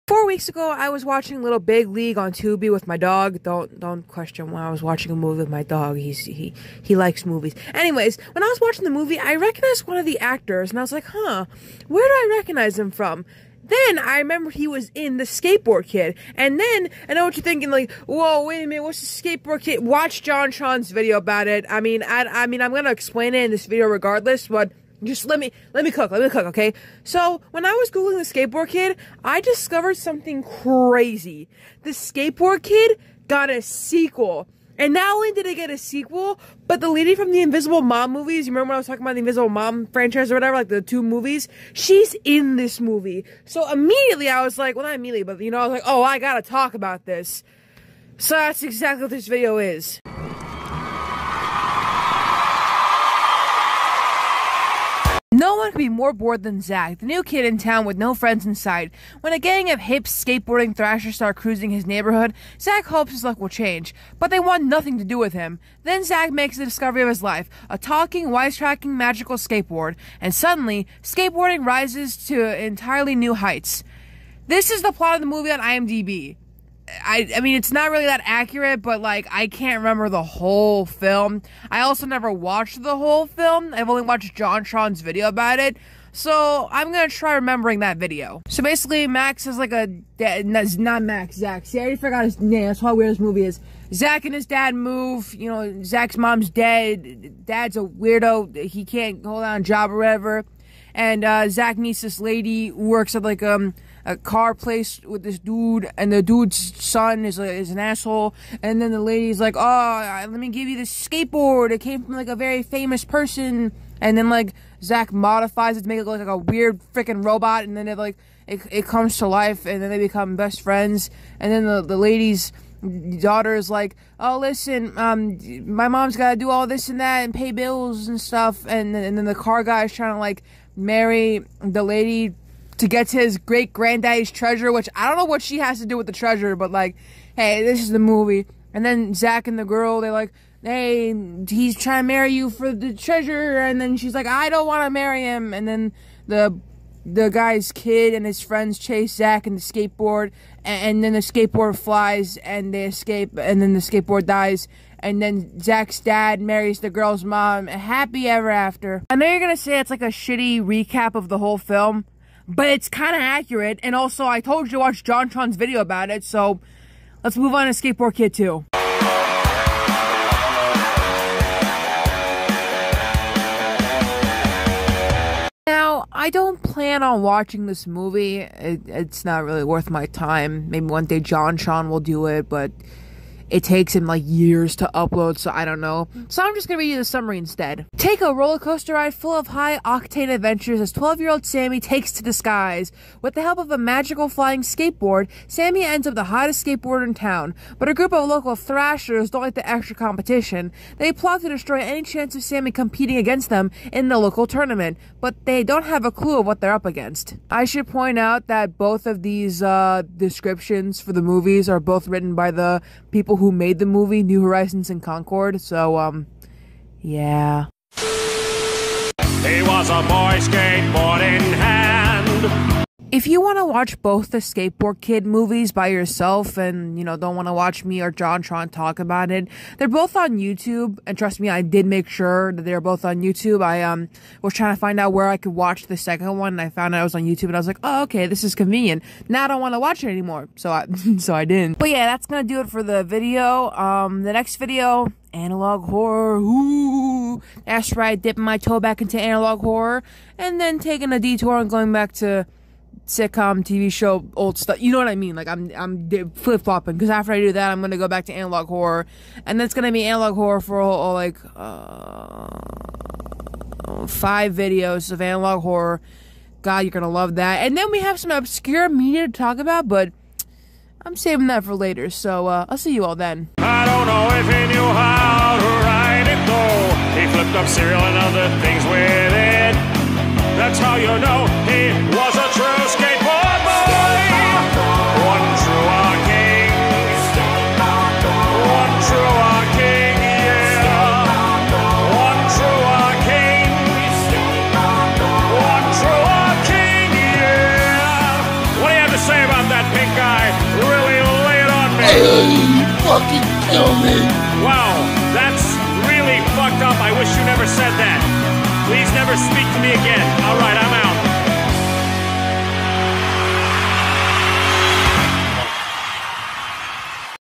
There's a sequel to the skateboard kid??
radical! today, I'll be talking about the skateboard kid and its totally tubular sequel!(I'm sorry for being cringe please don't unsubscribe)